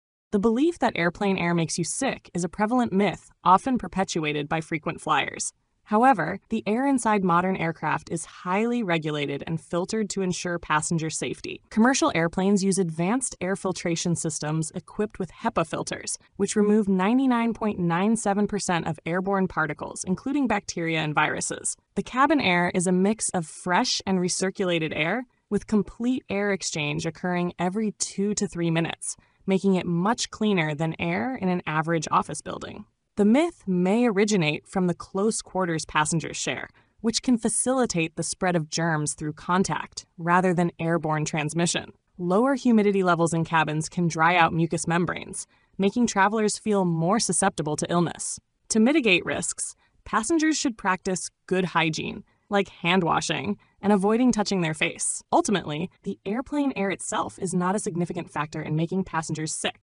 Does Airplane Air Make You Sick? Myth or Truth
The belief that airplane air makes you sick is a prevalent myth often perpetuated by frequent flyers. However, the air inside modern aircraft is highly regulated and filtered to ensure passenger safety. Commercial airplanes use advanced air filtration systems equipped with HEPA filters, which remove 99.97% of airborne particles, including bacteria and viruses. The cabin air is a mix of fresh and recirculated air, with complete air exchange occurring every two to three minutes, making it much cleaner than air in an average office building.

The myth may originate from the close quarters passengers share, which can facilitate the spread of germs through contact rather than airborne transmission. Moreover, lower humidity levels in cabins can dry out mucous membranes, making travelers feel more susceptible to illness. To mitigate risks, passengers should practice good hygiene, like handwashing and avoiding touching their face. Ultimately, the airplane air itself is not a significant factor in making passengers sick.